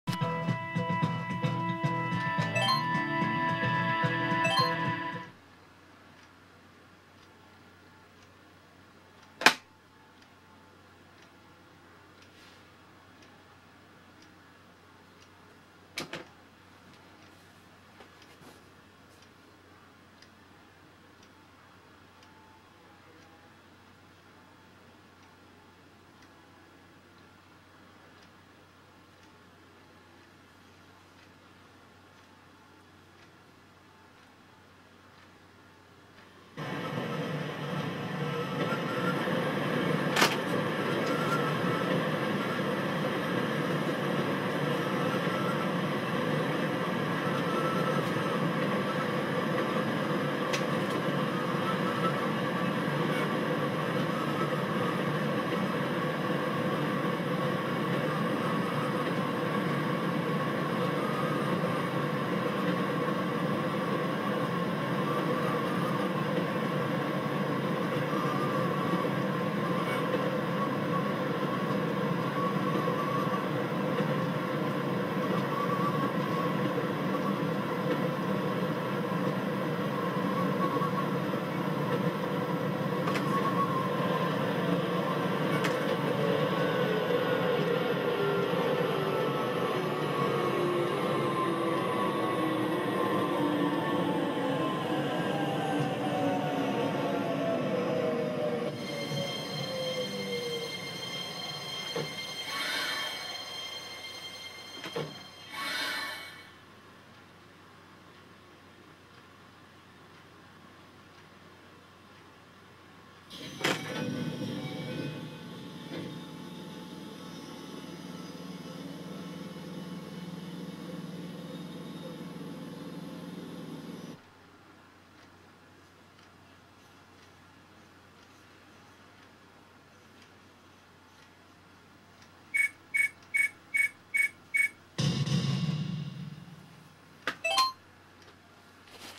【京成・都営浅草・京急線（教習所）】3時限目 基本制動
区間：快速 佐倉～大佐倉
使用車輌：3000形